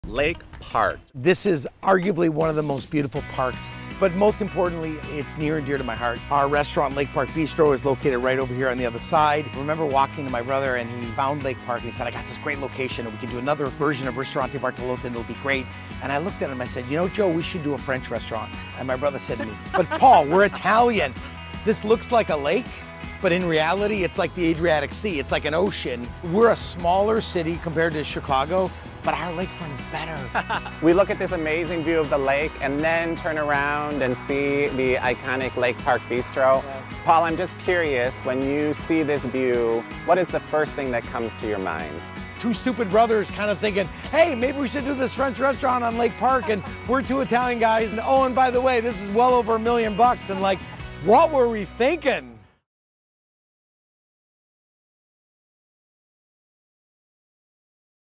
The Story Behind Lake Park Bistro in Milwaukee, Wisconsin
Good things are brewing in Milwaukee! Hear about chef and owner of Bartolotta Restaurants about how their French restaurant, Lake Park Bistro, came to be.